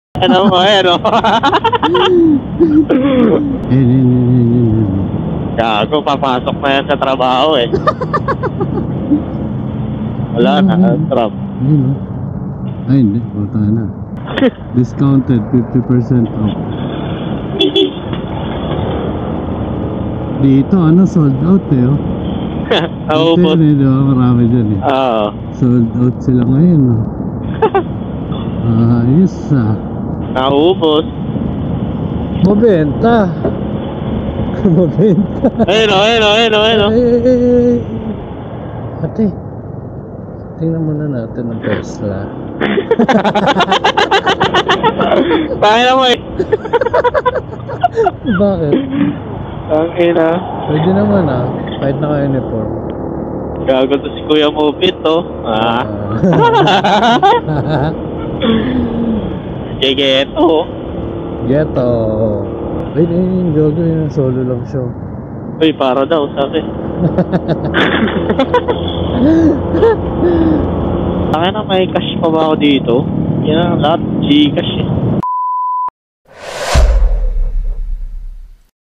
QUEZON AVE GIRLS | GUSTO ATA I TAKE HOME NI KUYA MOVE IT | BARAKO POWER | MCTAXI
si barako power gusto ata mag
take home ng item sa q. ave. HAHAHA

FOLLOW ME ALSO ON MY OTHER